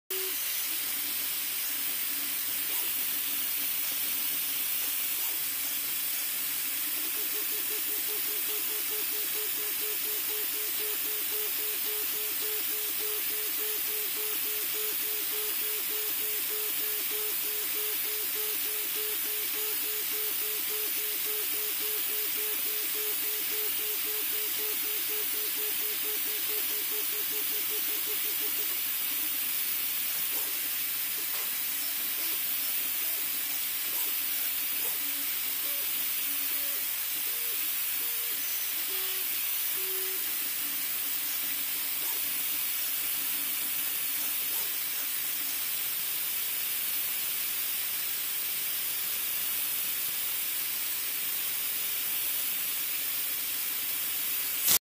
Printing Extrudr GreenTec @ 150mm/s
As an alternative to ABS i like GreenTec from Extrudr
It has nearly all the positive properties of ABS without the drawbacks.
It is similar in heat resistance and easily machineable, but different to ABS it does not warp and does not need an enclosure for bigger parts. On the downside it can not be smoothed using ABS, but on the upside it will fully biodegrade, much better than PLA.